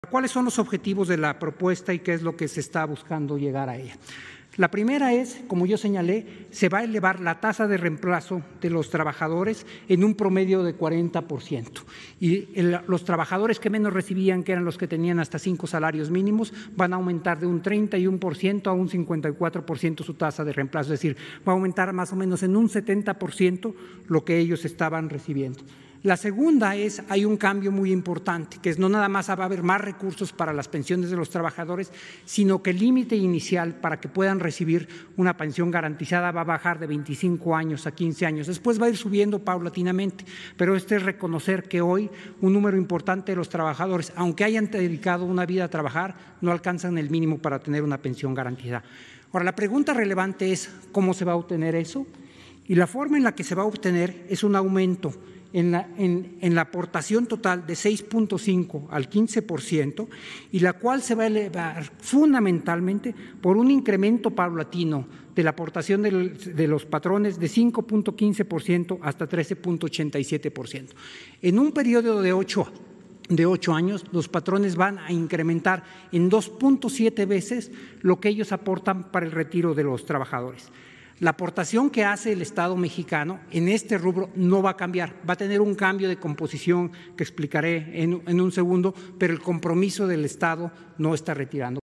El límite inicial para poder recibir una pensión garantizada va a bajar de 25 años a 15 años
Arturo Herrera Gutiérrez, Secretario de Hacienda y Crédito Público:

La primera es, como ya señalé, se va a elevar la tasa de reemplazo de los trabajadores en un promedio de 40 por ciento y los trabajadores que menos recibían, que eran los que tenían hasta cinco salarios mínimos, van a aumentar de un 31 por ciento a un 54 por ciento su tasa de reemplazo, es decir, va a aumentar más o menos en un 70 por ciento lo que ellos estaban recibiendo.

La segunda es que hay un cambio muy importante, que es no nada más va a haber más recursos para las pensiones de los trabajadores, sino que el límite inicial para que puedan recibir una pensión garantizada va a bajar de 25 años a 15 años y después va a ir subiendo paulatinamente, pero esto es reconocer que hoy un número importante de los trabajadores, aunque hayan dedicado una vida a trabajar, no alcanzan el mínimo para tener una pensión garantizada.